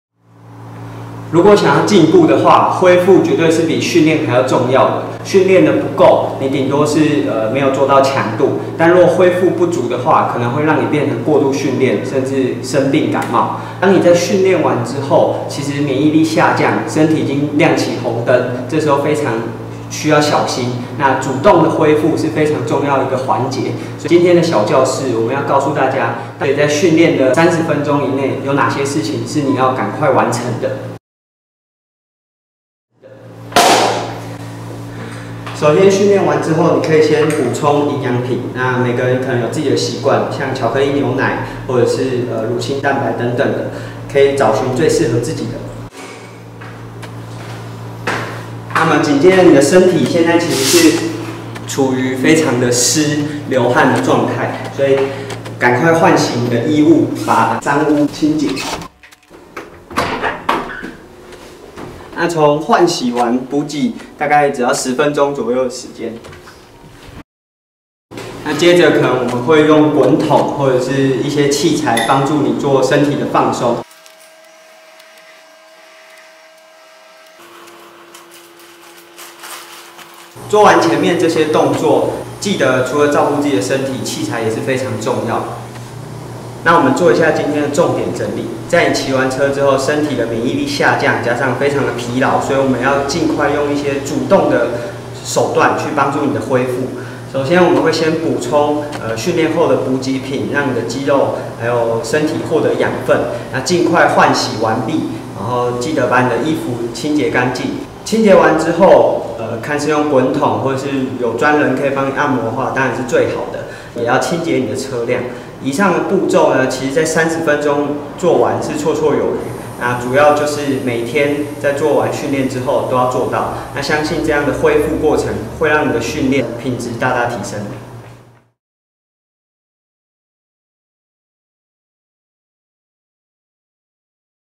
鐵人小教室 | 訓練過後 黃金30分鐘
完賽後必感冒？訓練後總覺得喉嚨不對勁？經過訓練後，身體免疫力下降，可能讓身體處於紅色警戒的狀態，但總看到訓練完、賽後穿著濕透的服裝，到處晃來晃去的夥伴，其實都是暴露在危險當中！

本期Keng_Sports鐵人小教室，一起了解訓練完之後的30分鐘，該怎麼做才能脫離危險，並讓訓練成效大大提升！